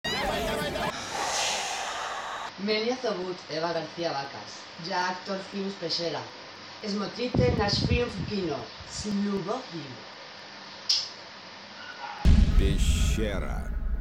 Актриса Ева из фильма ПЕЩЕРА передает поцелуи россиянам
Eva García-Vacas speaks russian well :)

Ева Гарсия-Вакас снималась в 8-ми фильмах и сериалах.

Она Вам еще покажет. В Пещере!